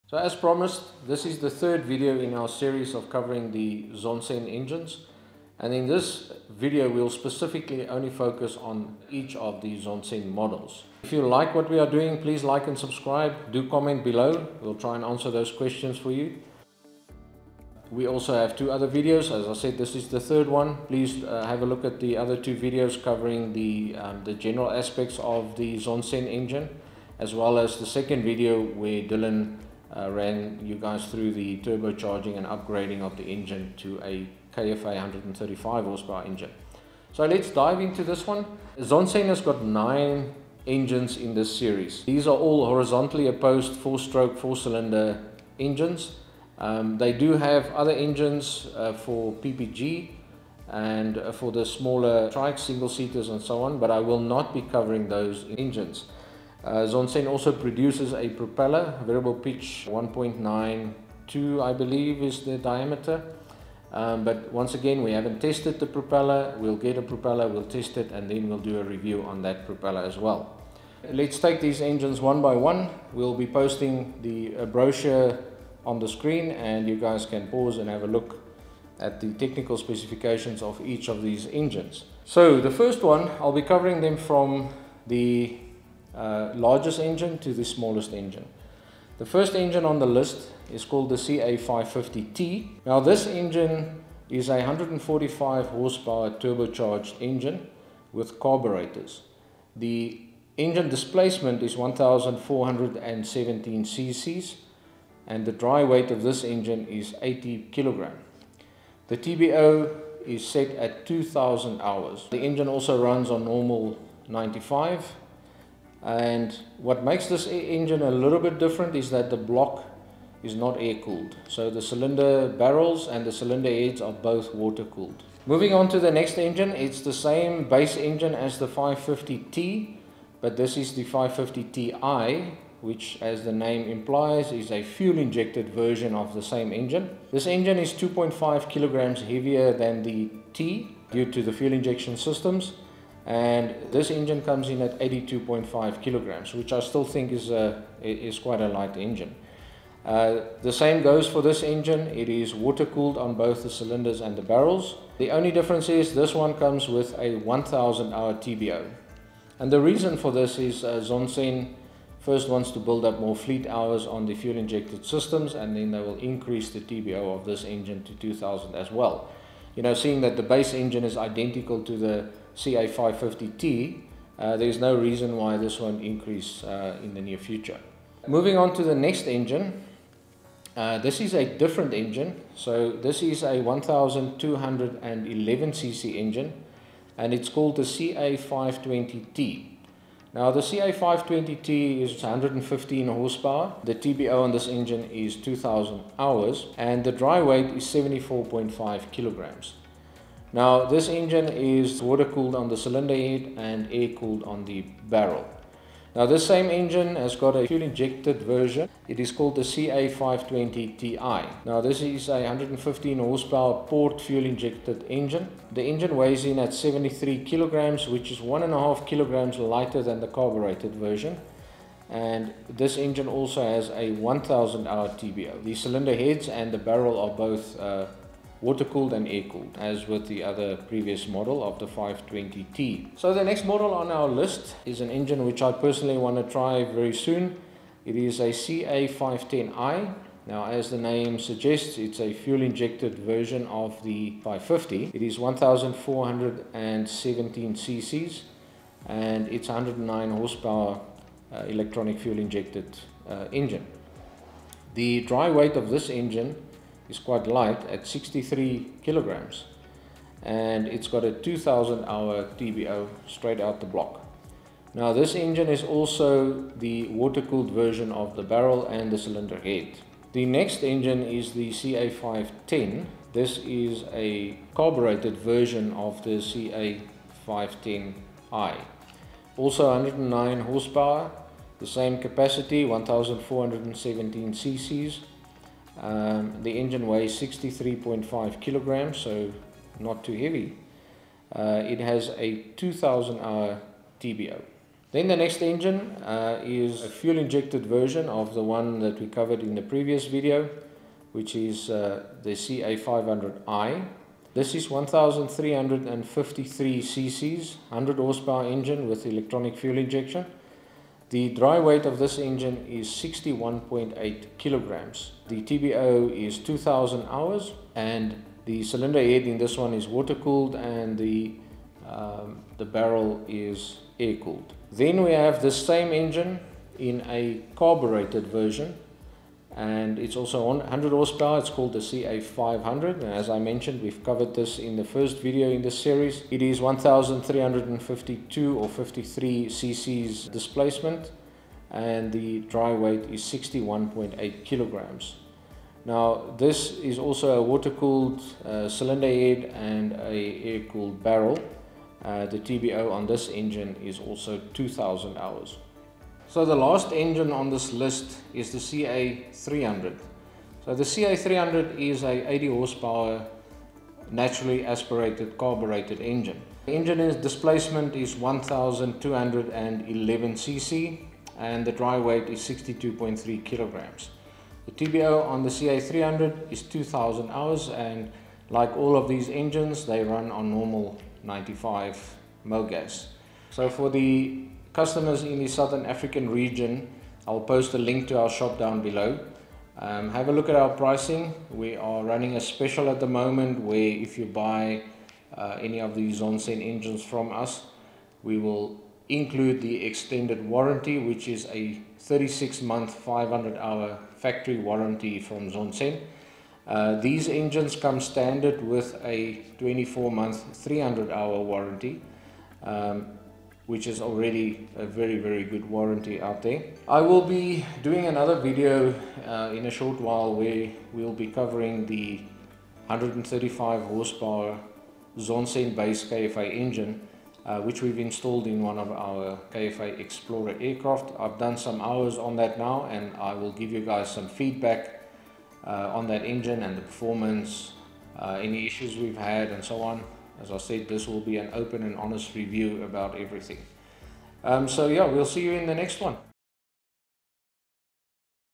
ZONSEN engine models | a complete breakdown
In Episode 3 of the Zonsen engine series we take an in-depth look at the different Zonsen Aero engine models.